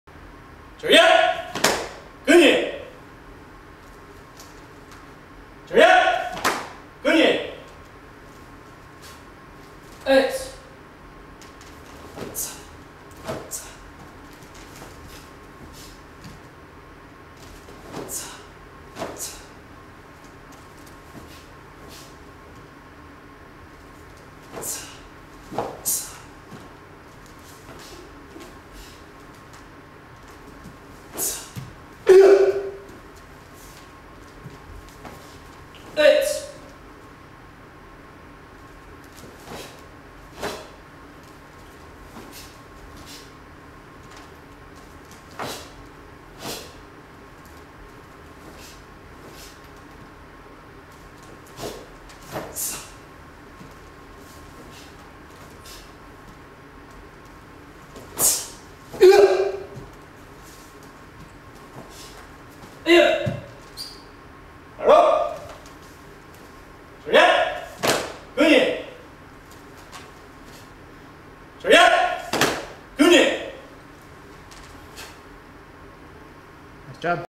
2nd Degree Brown Kicking Series
Self defense series required for testing in the Karate North - Tae Kwon Do system.

One attacks, the other defends, then they switch.

These are just some of many combination's that can be done to defend against an attack.